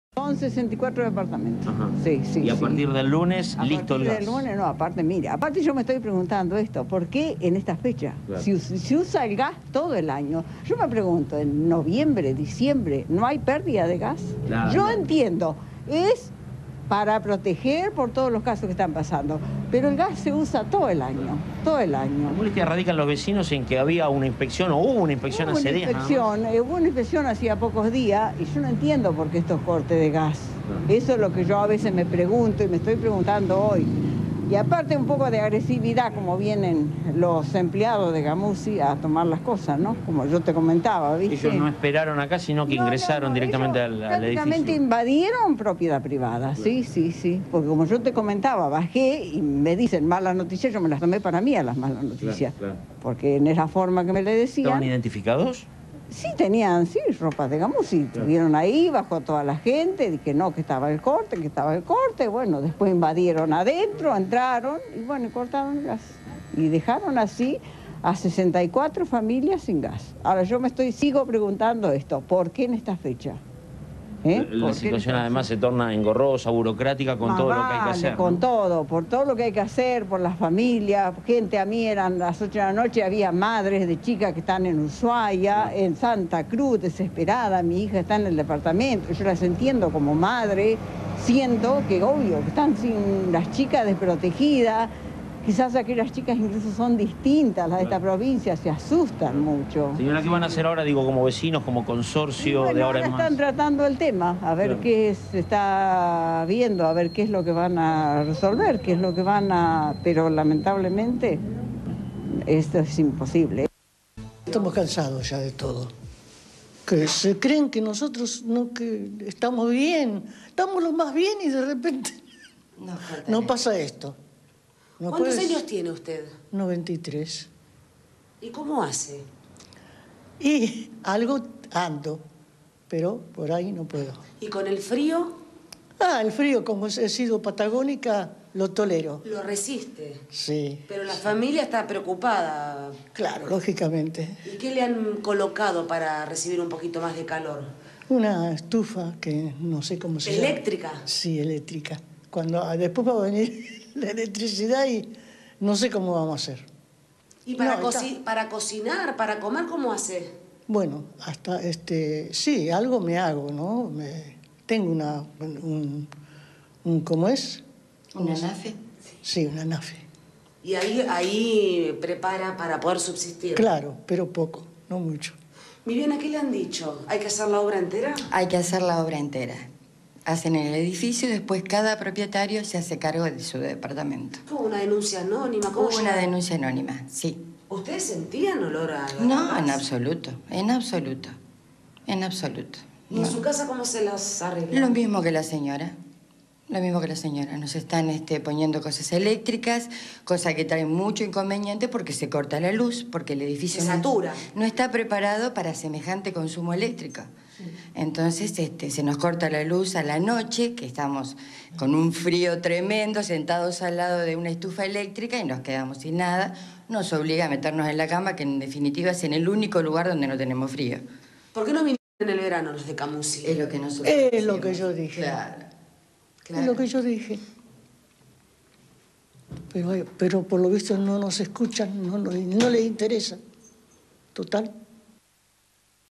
Quejas contra Camuzzi: "¿Por qué no cortan el gas en verano?"
En Bahía hay más de 20 edificios sin gas por supuestas fallas en los medidores.
Video: canal 7